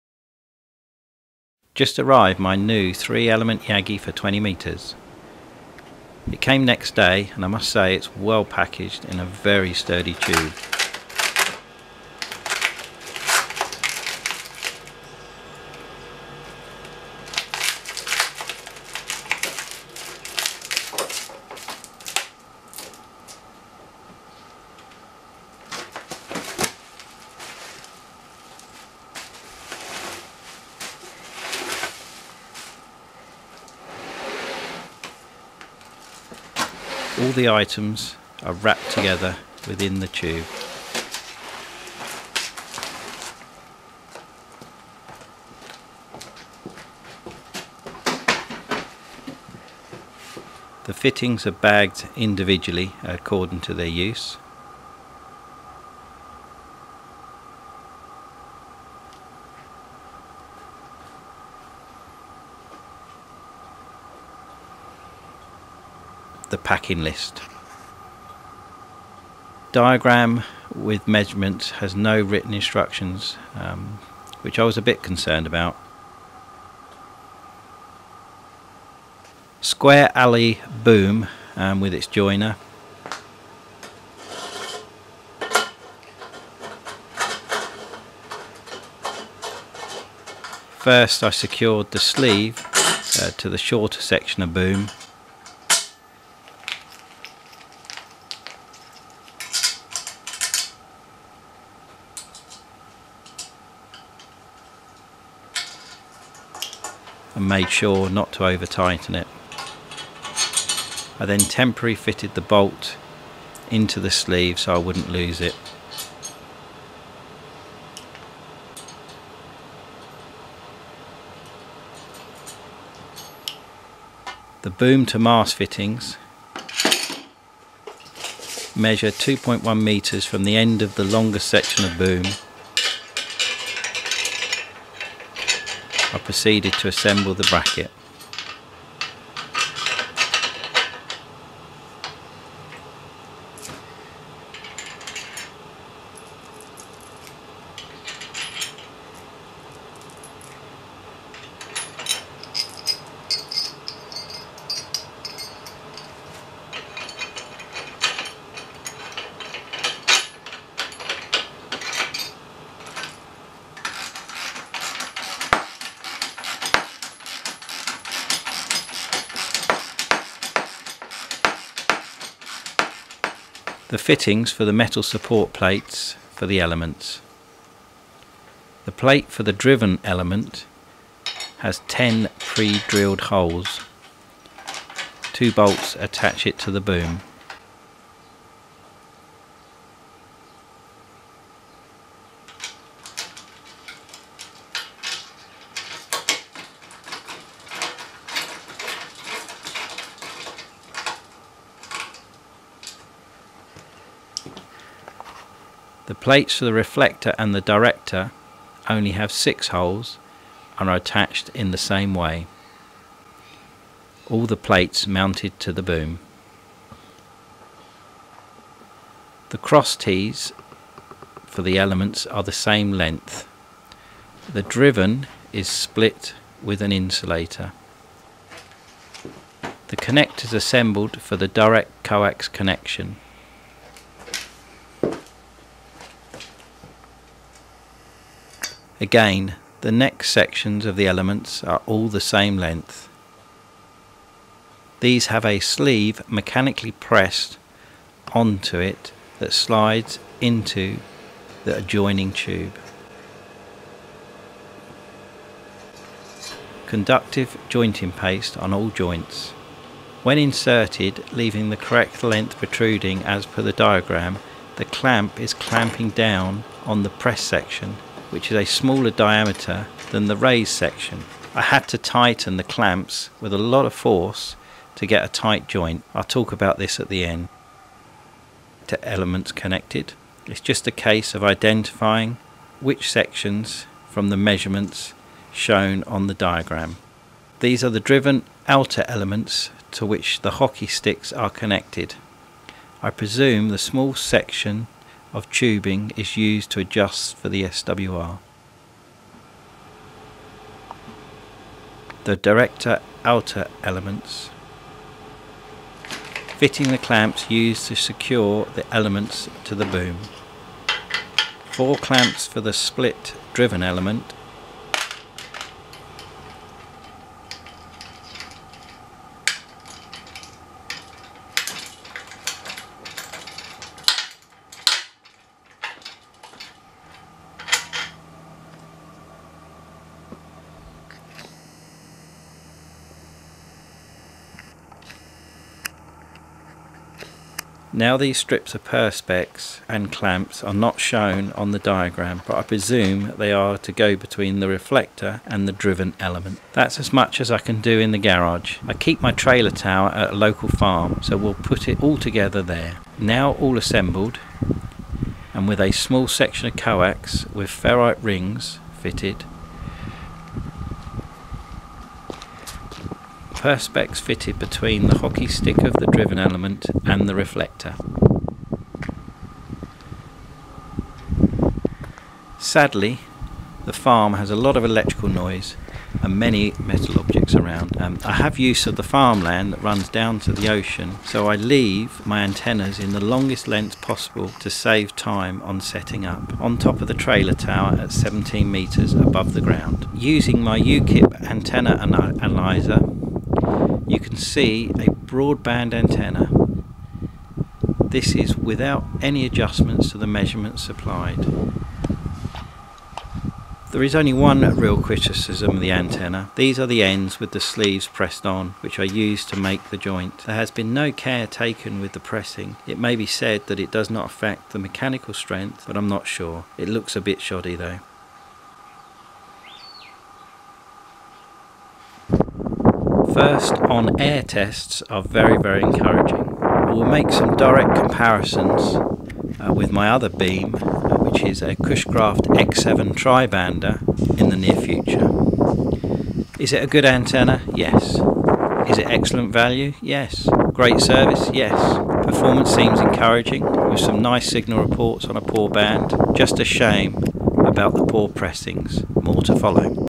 InnovAntennas 14MHz OP-DES 3 Element Yagi Beam Un-boxing and assembly. Ham Radio
My new mono-band 3 element yagi for 20M. Un-boxing and assembly.
Installed on my portable trailer tower at 17m for testing. Ham Radio